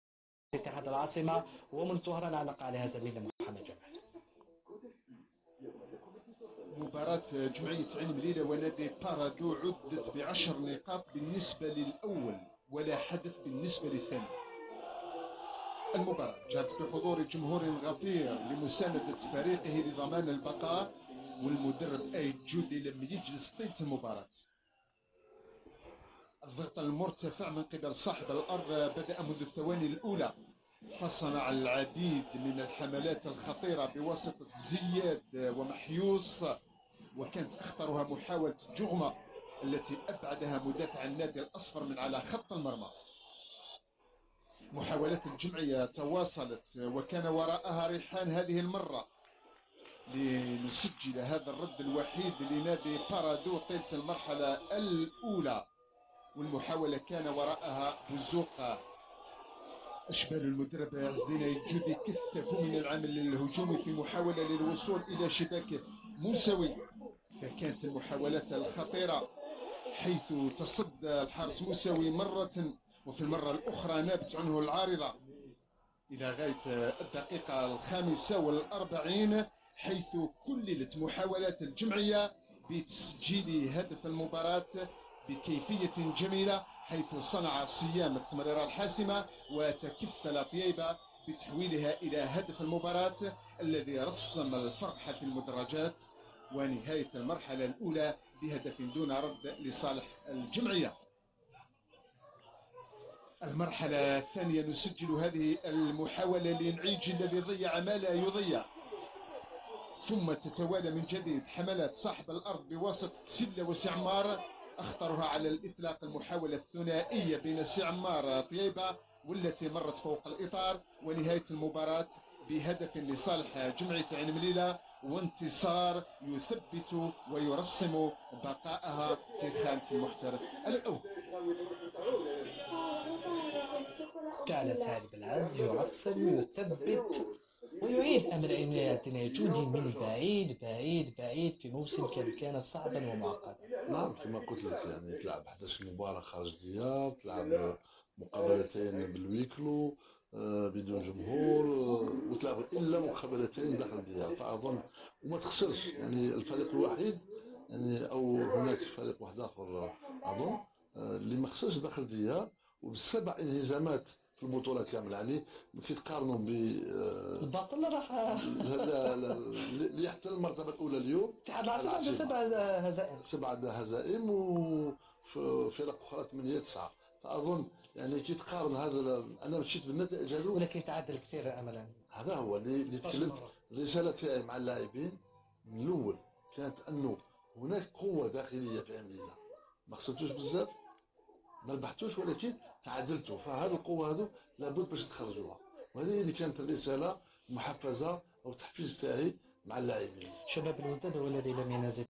ج عين مليلة 1 ن بارادو 0
Résumé du match ASAM 1 Paradou AC 0